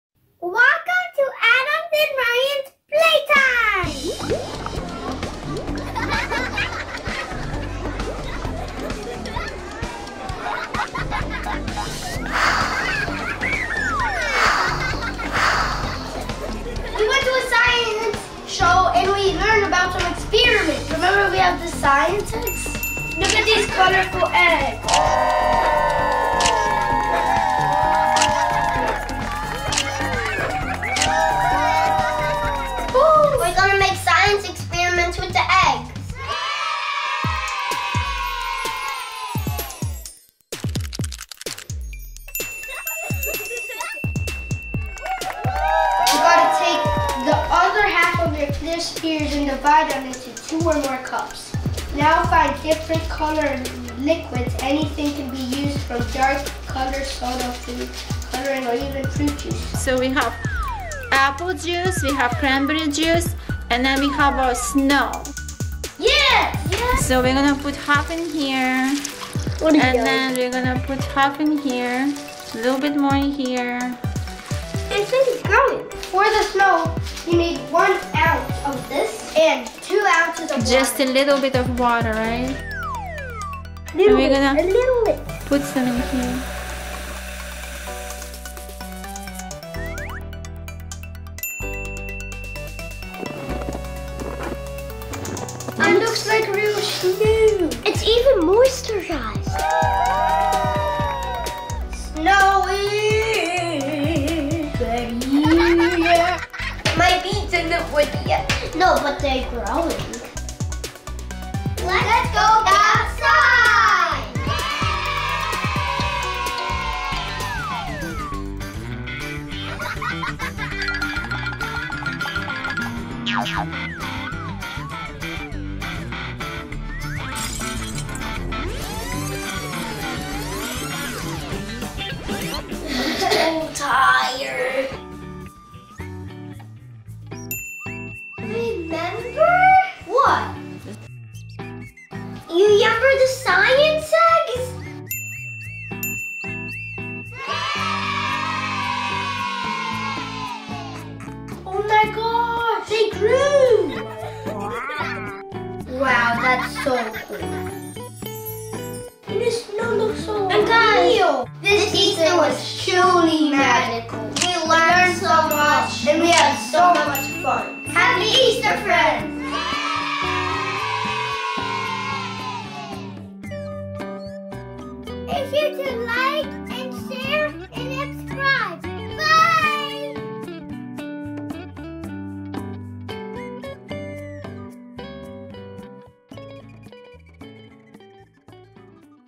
Unbelievable Results from these Mad Science Experiments!
​ @AdamAndRayanBros, same way as most kids out there, love easter holiday and surpriseeggs, but these eggs were way more than regular! What contained on those eggs wasn't just candy, but a cool Mad Science Surprise! In fact they loved those science eggs so much that event participated in the madscience show in their school where kids were participating and doing coolest fire, steam, water and smoke crazy science experiments with the coolest scientists ever!

⬇️ Follow us ⬇️
Facebook : Adam and Rayan Family Adventures